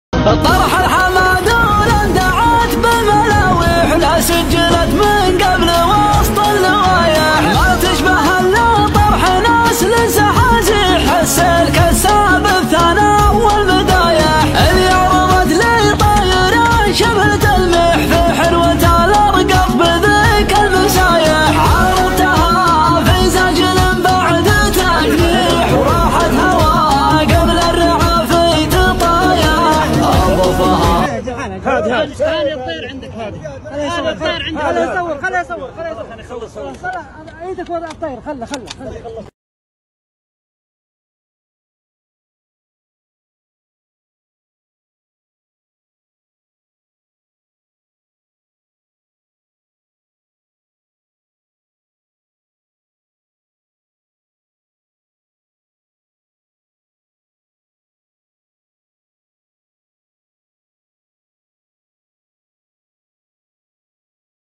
لحظة طرح فرخ شاهين تام طرح الجفر الاردن 2022/10/18 طرح اليوم
طرح الجفر الاردن فرخ شاهين تام 2022/10/18
فرخه تامه طرح مقناص الجفر 18/10مع حيدر العودات
مزاد الفرخه التامه طرح مقناص الجفر  مع القناص حيدر عبد العزيز العودات  الساعه 3/30 عصر اليوم  الموقع الغليضه عزبه ابو فيصل العودات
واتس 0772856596
حيدر عبد العزيز ابو راشد
تقميط ابحريه
شيهانه طرح الاردن